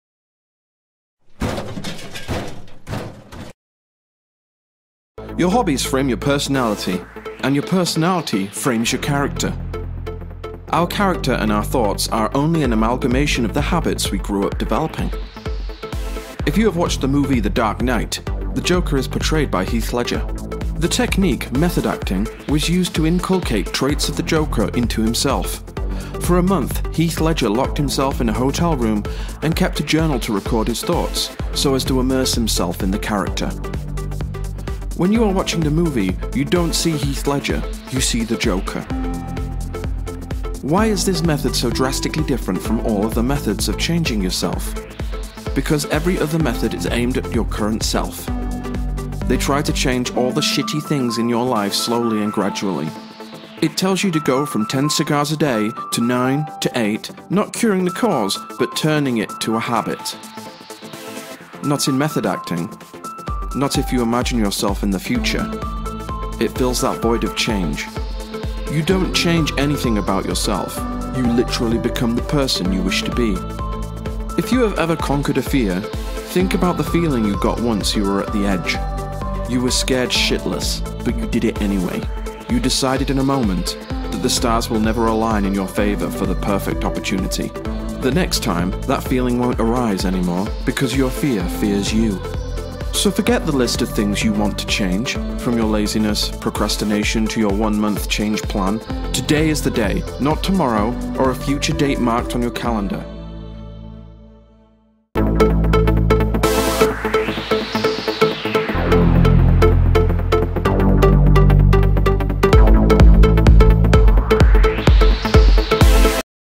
Method Acting - Making Changes
Your hobbies frame your personality and your personality frames your character.
Our character and our thoughts are only an amalgamation of the habits we grew up developing.
If you have watched the movie, The Dark Knight, The joker is portrayed by Heath Ledger.  The technique, “method acting” was used to inculcate traits of the joker into himself. For a month, Heath Ledger locked himself in a hotel room and kept a journal to record his thoughts, so as to immerse himself in the character.
When you are watching the movie, you don’t see Heath Ledger, you see the joker.
Why is this method so drastically different from all other methods of changing yourself? Because, every other method is aimed at your current self. They try to change all the shitty things in your life slowly and gradually. It tells you to go from 10 cigars a day to 9, to 8, not curing the cause but turning it to a habit.
Not in method acting. Not if you imagine yourself in the future. It fills that void of change. You don’t change anything about yourself. You just literally become the person you wish to be.
If you have ever conquered a fear, think about the feeling you got once you were at the edge. You were scared shitless, but you did it anyway. You decided in a moment, that the stars will never align in your favor for the perfect opportunity. The next time, that feeling won’t arise anymore because your fear fears you.
So forget the list of things you want to change,  from your laziness, procrastination to your 1 month change plan. Today is the day! Not tomorrow, or a future date marked on your calendar.